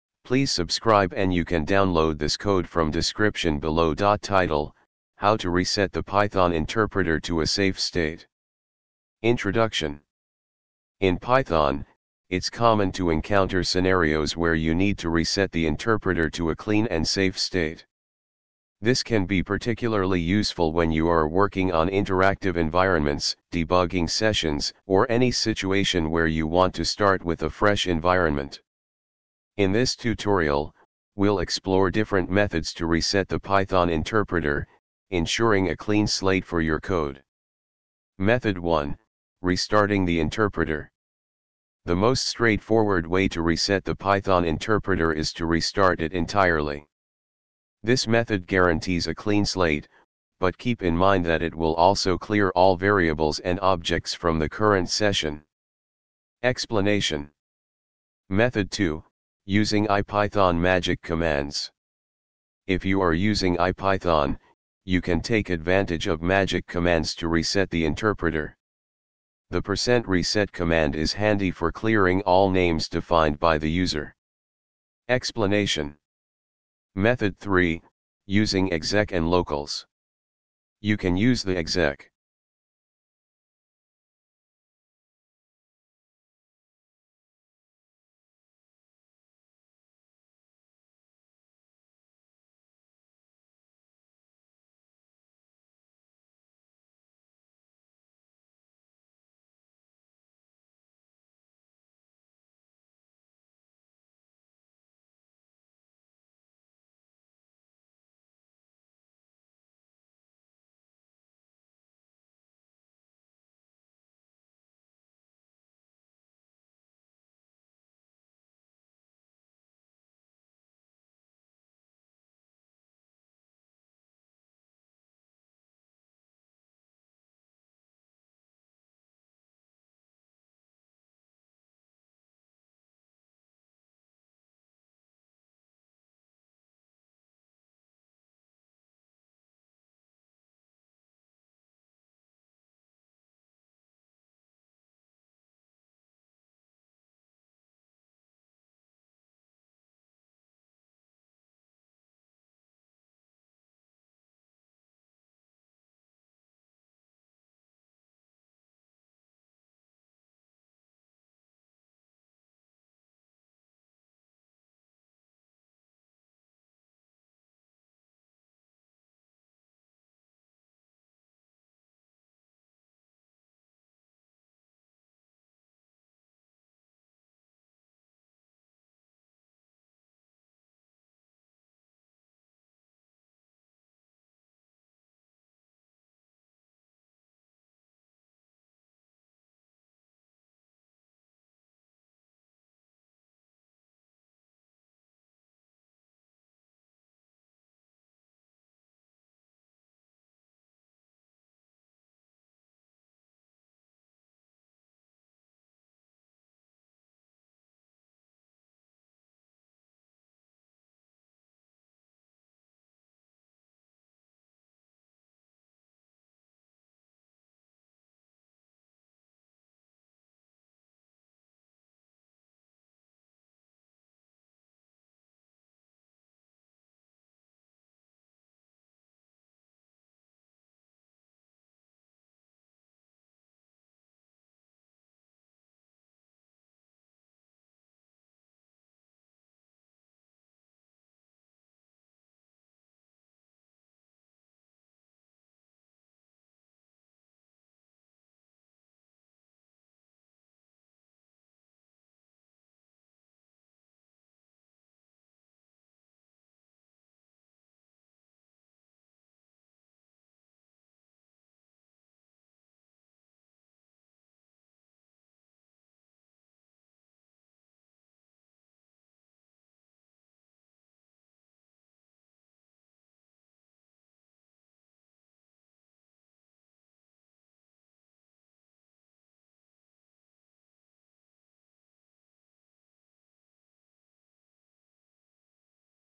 How to reset Python interpreter to a safe state
Title: How to Reset the Python Interpreter to a 'Safe' State
Introduction:
In Python, it's common to encounter scenarios where you need to reset the interpreter to a clean and safe state. This can be particularly useful when you are working on interactive environments, debugging sessions, or any situation where you want to start with a fresh environment. In this tutorial, we'll explore different methods to reset the Python interpreter, ensuring a clean slate for your code.
Method 1: Restarting the Interpreter
The most straightforward way to reset the Python interpreter is to restart it entirely. This method guarantees a clean slate, but keep in mind that it will also clear all variables and objects from the current session.
Explanation:
Method 2: Using IPython Magic Commands
If you are using IPython, you can take advantage of magic commands to reset the interpreter. The %reset command is handy for clearing all names defined by the user.
Explanation:
Method 3: Using exec and locals()
You can use the `exec